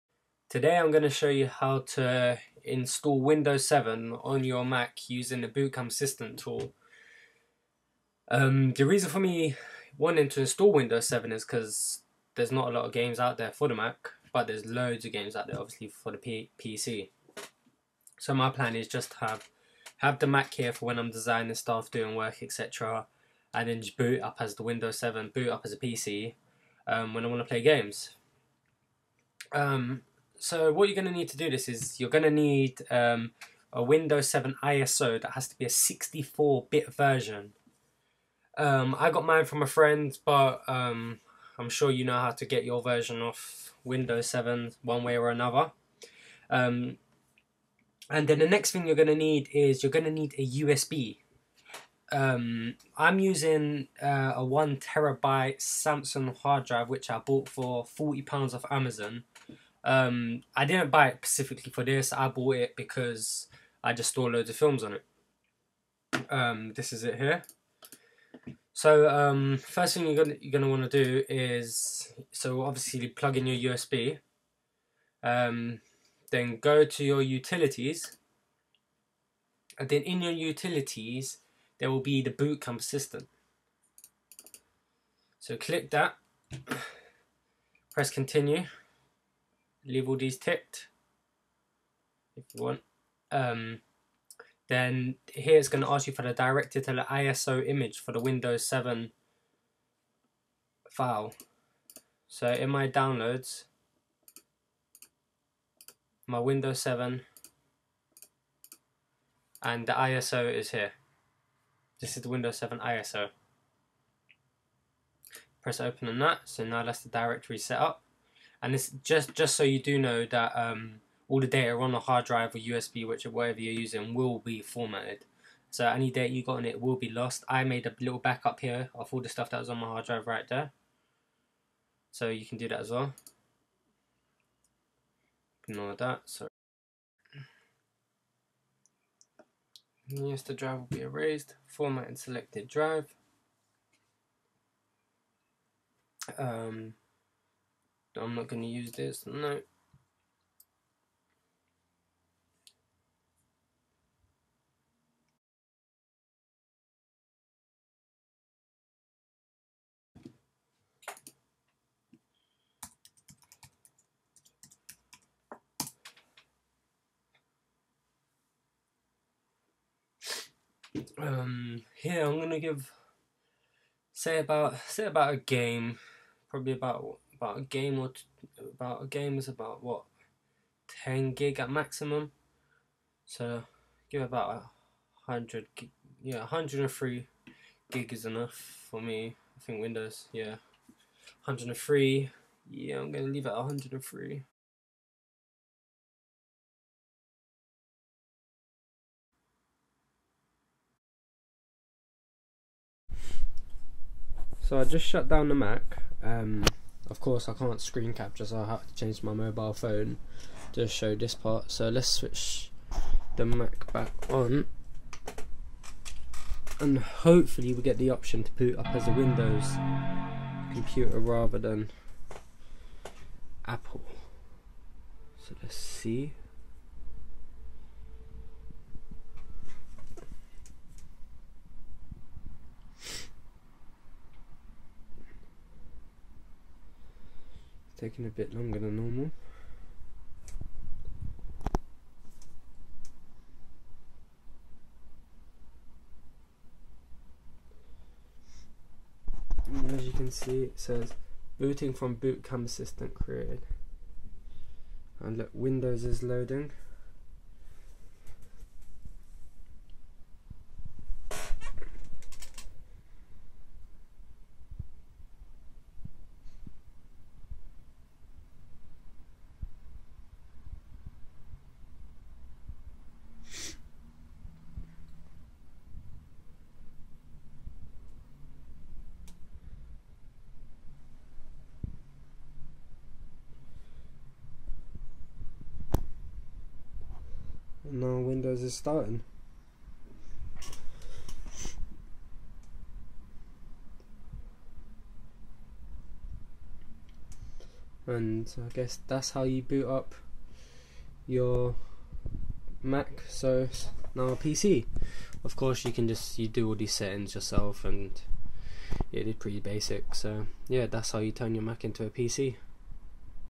Turn Your Mac Into A PC
Using the boot Camp assistant tool I show you how to install windows 7 onto your mac. Once you have installed your version of windows 7 you now have the option to boot up your mac using OSX or Windows whenever you like.

I chose not go with Parallels or VM Fusion as it divided power between your mac and the software where as using bootcamp you can use the full potential of your mac including 100% GPU, 100% Gpu and 100% of the RAM so its ideal for gaming.